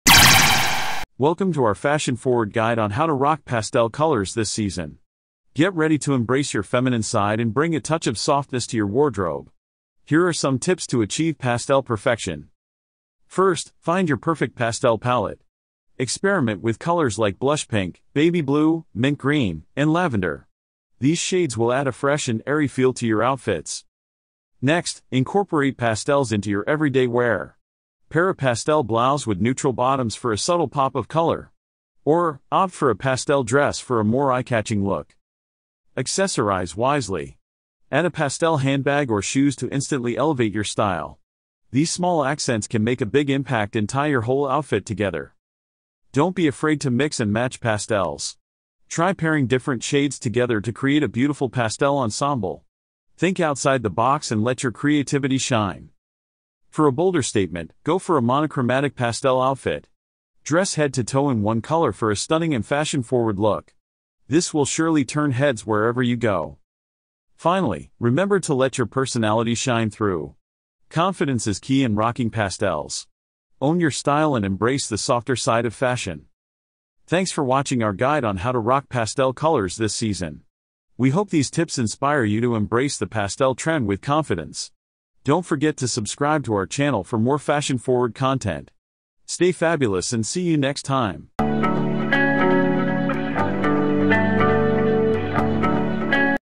Pastel Perfection: Fashion Forward Tips to Rock This Season's Colors
Are you ready to rock the latest trend in fashion? Get ready to embrace pastel colors this season and elevate your style game! In our exciting video, "Pastel Perfection: Fashion Forward Tips to Rock This Season's Colors," we've curated a fabulous collection of ideas and inspiration for women's clothing.

Discover the secrets of effortlessly incorporating pastels into your wardrobe, turning heads wherever you go. Our fashion experts will guide you through a variety of stunning outfits that perfectly showcase the charm and versatility of these soft hues. From pretty pastel dresses to chic pastel tops paired with trendy bottoms, we've got you covered.

Learn how to mix and match different pastel shades to create unique and eye-catching looks that complement your complexion and personal style. Whether you're aiming for a soft and feminine aesthetic or a bold and playful vibe, we'll share tips on accessorizing and styling to complete your perfect outfit.

But wait, there's more! We'll also introduce you to the latest trends in pastel-colored accessories, such as handbags, shoes, and jewelry, to add that extra touch of elegance and sophistication to your ensembles.

So, why wait? Join us on this exciting fashion adventure and master the art of rocking pastel colors this season. Elevate your fashion game and become the epitome of style and grace. Stay ahead of the curve with our expert advice and unlock a world of endless possibilities.

Don't miss out on this incredible opportunity to revamp your wardrobe with the freshest pastel looks.